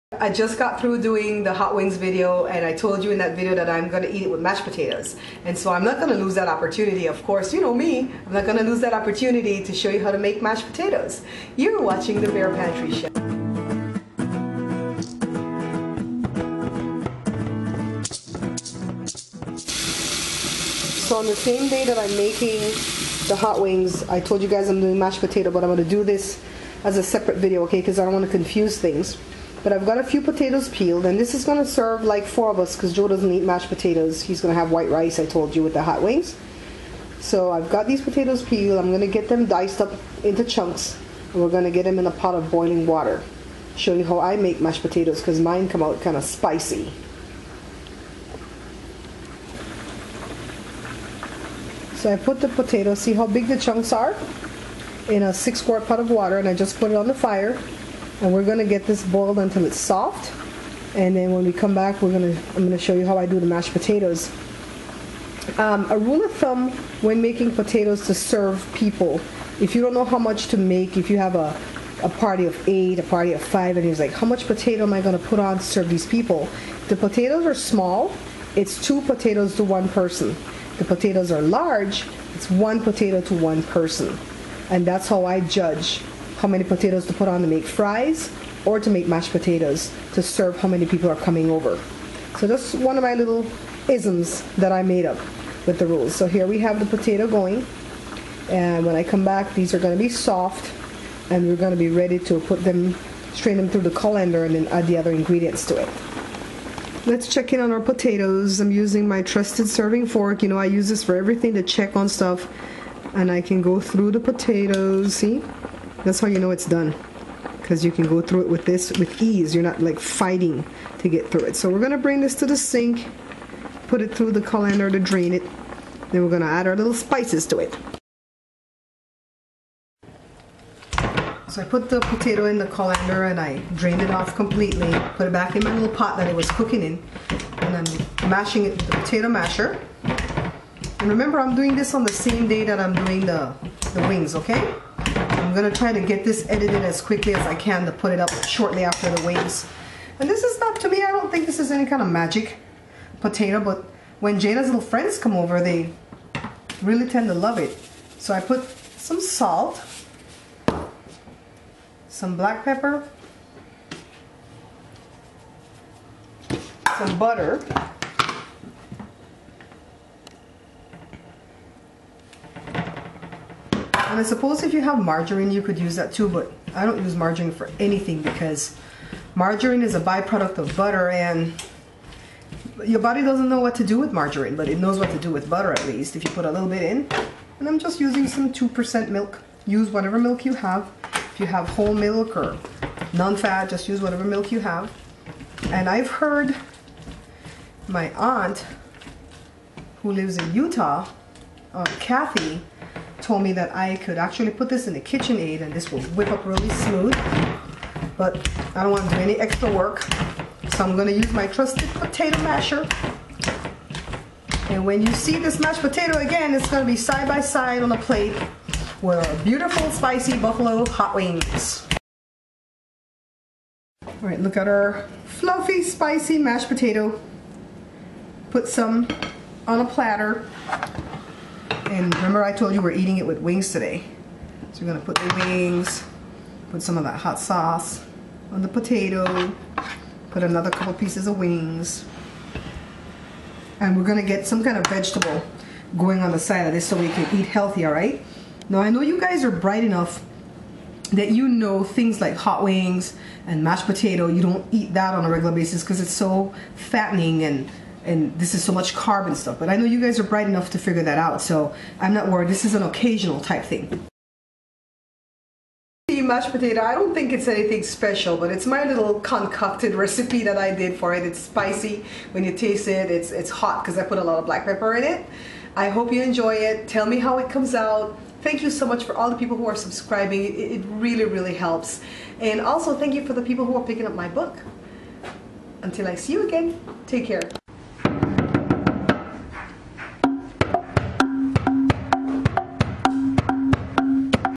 Spicy Mashed Potatoes
These mashed potatoes are fluffy and spicy. If you want it less spicy, just use a little less black pepper. I use only real butter. I use Russet, Idaho, or Irish potatoes to make mash potatoes, but it is also delicious when made with sweet potatoes.